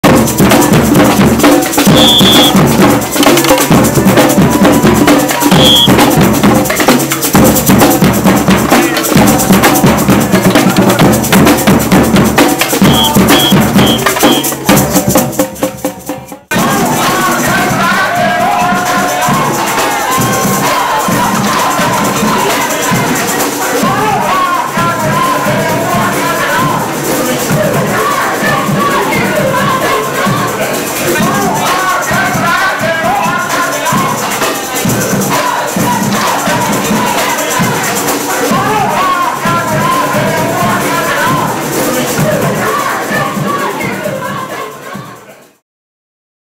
ERIC CANTONA - RITZY LIVING WAGE SUPPORTER
On their 6th day on strike Ritzy staff return to Hackney for an Amnesty International film screening. To chants of 'Oo Ah Cantona' the man himself went inside the Hackney Picturehouse for a Q&A about his new film, but seconds later he was seen on the protest shaking hands with staff and soaking up the lively samba rhythms provided by the fantastic 'Rhythms of Resistance'.